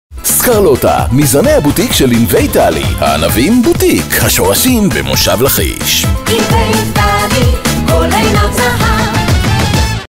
סקרלוטה - זן הענבים החורפי של ענבי טלי (10 ש)
הכירו את זן הענבים החורפי של ענבי טלי -  הסקרלוטה.

ענבי טלי ממושב לכיש שלרוב מזוהים עם עונת הקיץ, משיקים לקראת עונת החורף הקרובה את ענבי סקרלוטה, זן שגדל, מבשיל ונבצר בעונת החורף

ומשווק לחנויות כשהוא במצב טרי ממש כמו בקיץ.

בנוסף, הסקרלוטה מזוהה עם מרקמו הפציח, טעמו המתקתק, סגולותיו הרבים ויכולתו לשמש כתחליף מתוק לחטיף עם כ-60 קלוריות ל-100 גרם.

מומלץ לשלב ענבי סקרלוטה כתוספת לסלטים ועוגות :)

קרדיטים:
שיקו דומוביץ', מנכ"ל ענבי טלי
ליאו ברנט ישראל, חברת פרסום בינלאומית